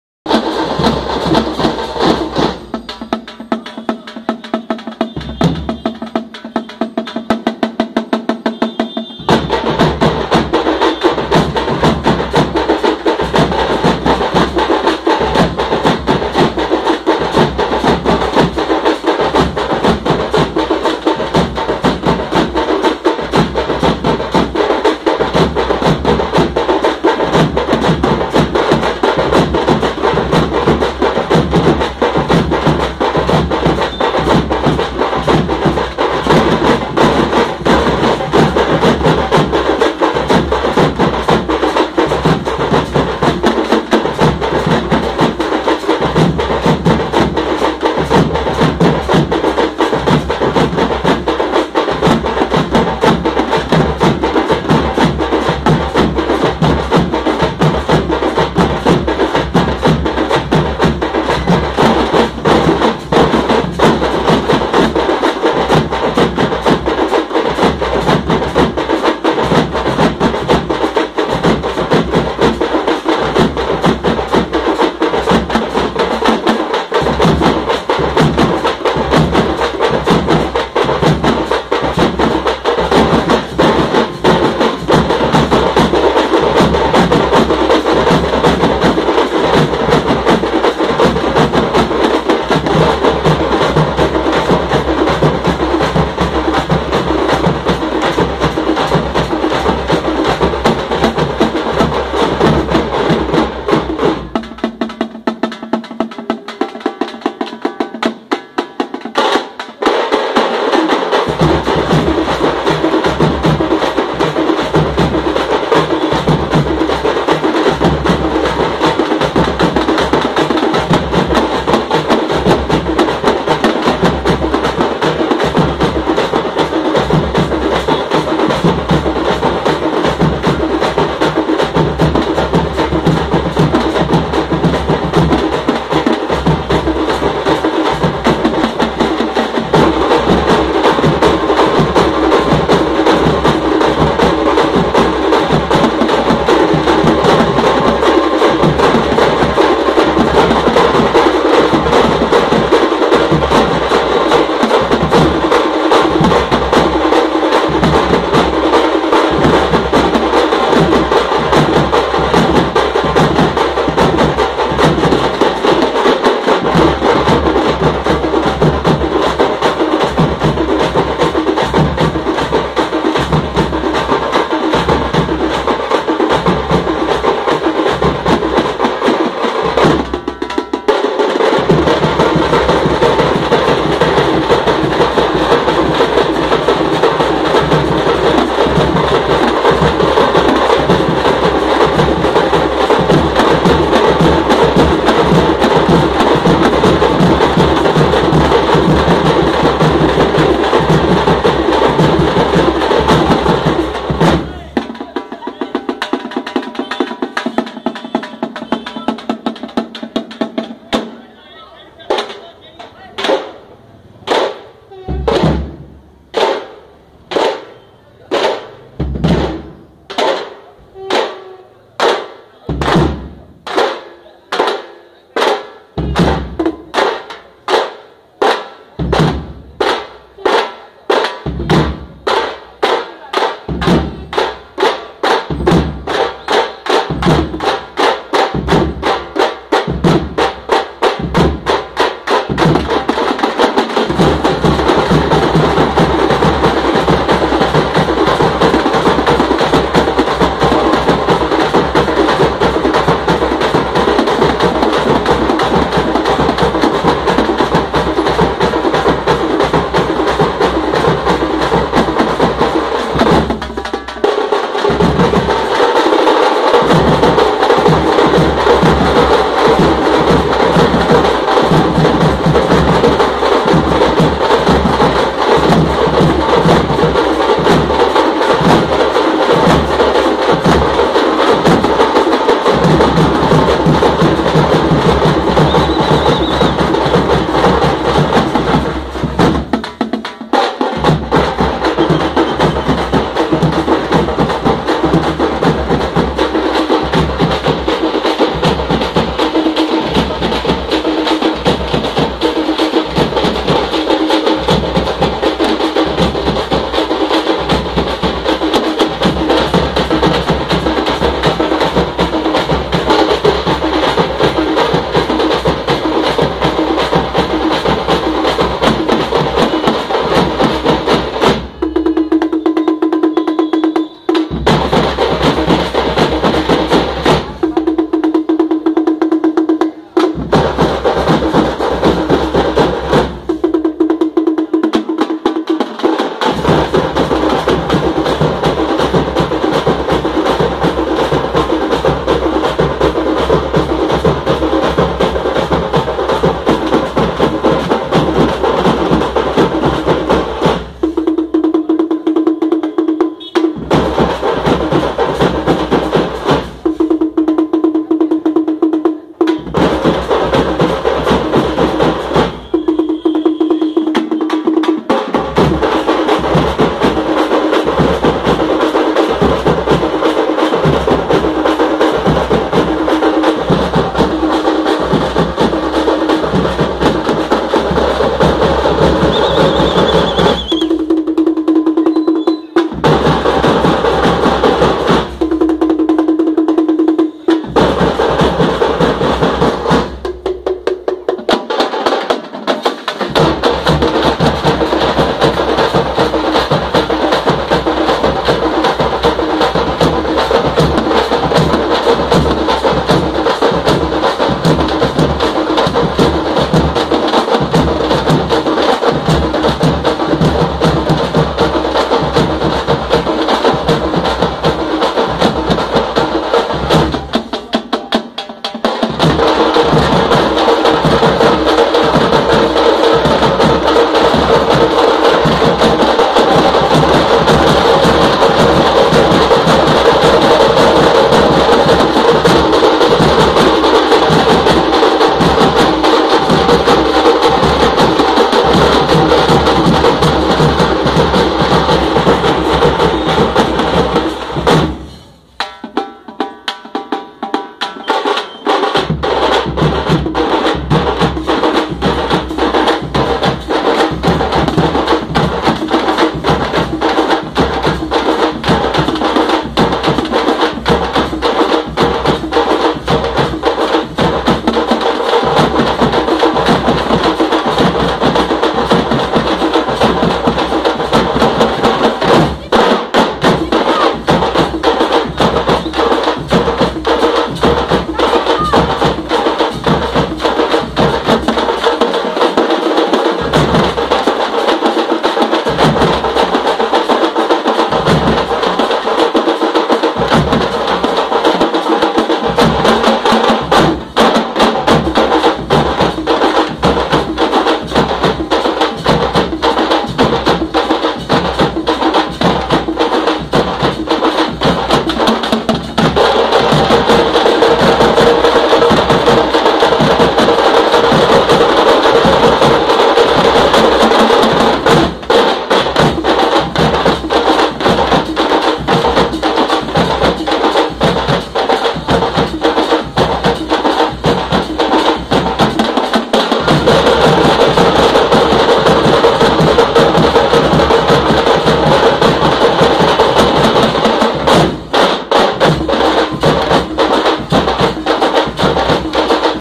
TAMTE Indian Instruments -South Indian/ Indian Metal-HD-YOU NEED TO WATCH TILL THE END ;)
south indian speciality instrumental music which makes your mind go crazy, you can never judge this unless you lisend to it

watch the next part by clicking the link below-

part 3-